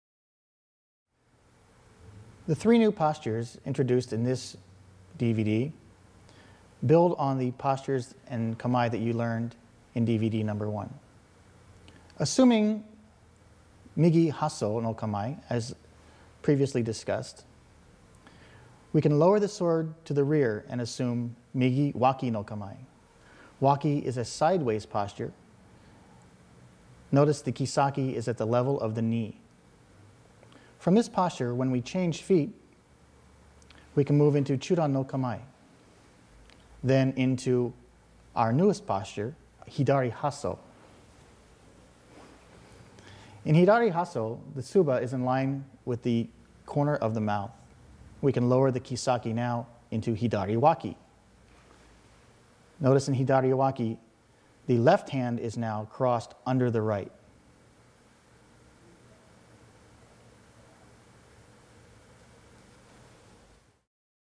Masayuki Shimabukuro Samurai Swordsmanship: Postures [3/10] CD2
black belt Hall presents: the art of sword of the samurai in the famous Wizard Masayuki Simabukuro (Masayuki Shimabukuro)-Weapons Instructor of the year, 2006-and his senior disciple Karl e. long (Carl e. long) that demonstrate rituals and technology tell us about the history and construction of the sword of the Samurai. This trehtomnaa DVD collection includes the basics of Iaido, intermediate, and advanced level, as well as interviews with both teachers.